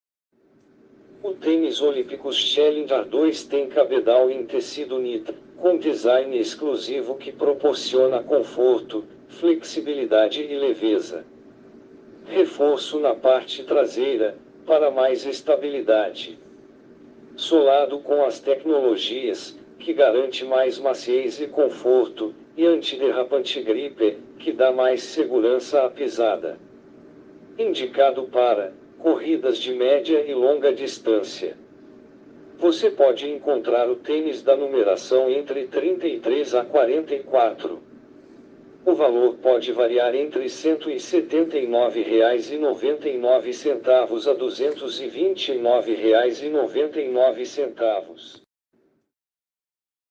CONHEÇA O TÊNIS OLYMPIKUS CHALLENGER 2 MASCULINO - MODELOS EM TRÊS CORES.
O Tênis Olympikus Challenger 2 tem cabedal em tecido Knit, com design exclusivo que proporciona conforto, flexibilidade e leveza. Reforço na parte traseira, para mais estabilidade. Solado com as tecnologias, que garante mais maciez e conforto, e antiderrapante Gripper, que dá mais segurança à pisada.

INDICADO PARA: Corridas de média e longa distância.

Você pode encontrar o tênis da numeração entre 33 a 44.

O valor pode variar entre R$ 179,99 a duzentos e vinte e nove reais e noventa e nove centavos.